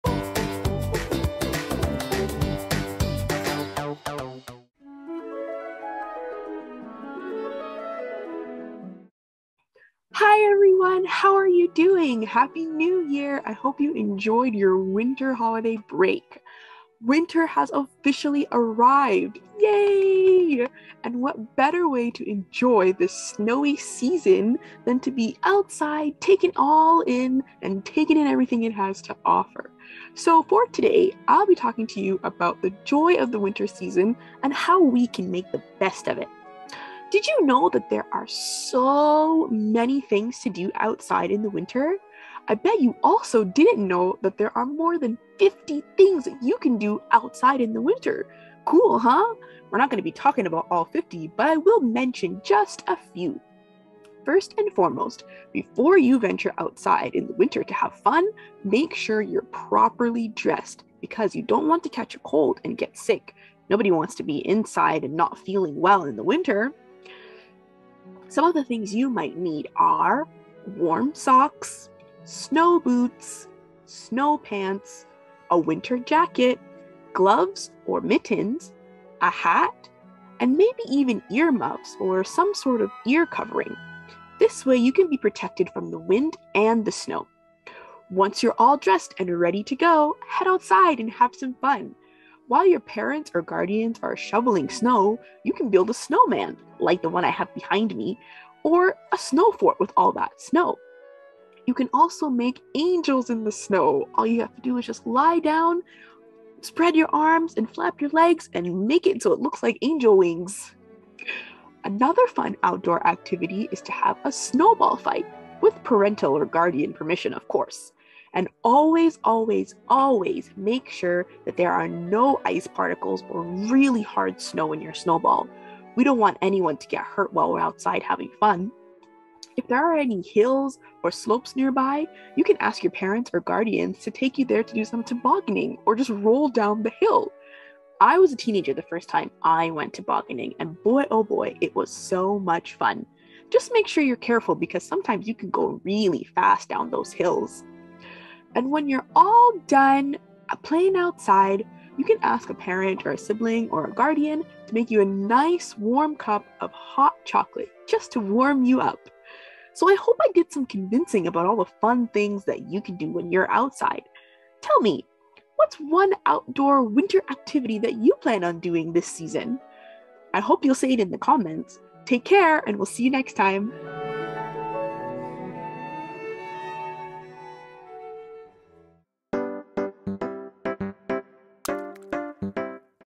Learning to Love Winter for Kids! with Shauna-Marie
Looking for more reasons to enjoy winter for the kids? Shauna-Marie can help us out with that!  Enjoy her list of all the fun and awesome things the winter season brings.

Did you enjoy this book and want to share with us how it went?  Or, do you have any activity or book reading requests?